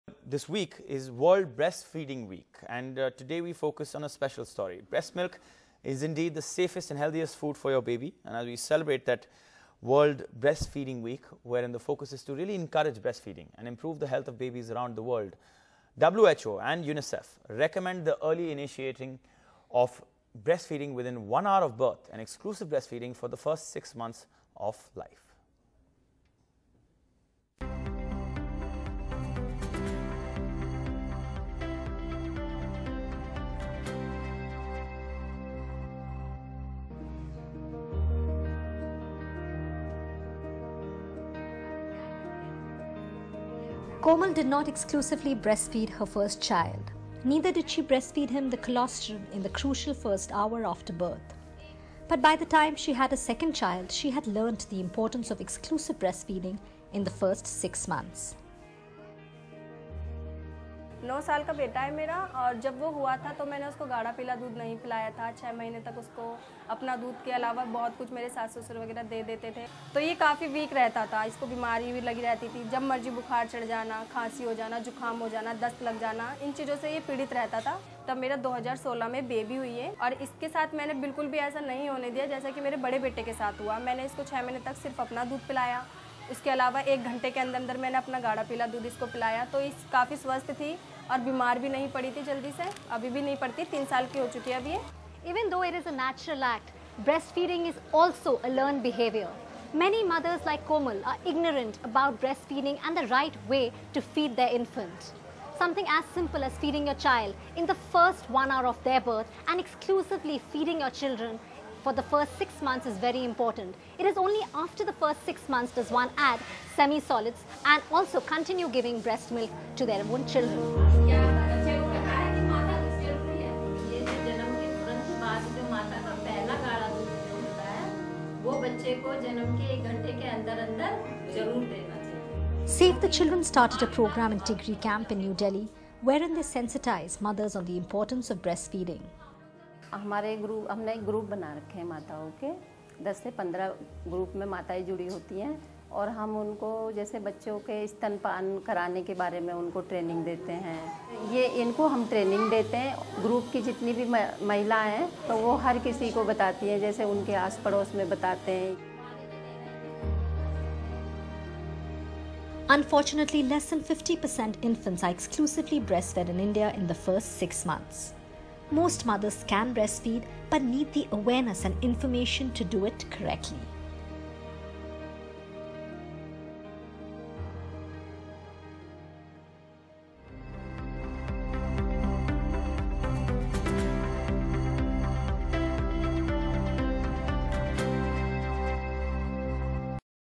Sensitizing Mothers On The Importance Of Breastfeeding
Lack of knowledge on exclusive breastfeeding and feeding the child in the first hour of birth is very crucial. As we celebrate World Breastfeeding Week from August 1 to August 7, we try to encourage breastfeeding among new mothers that will help improve the health of babies around the world.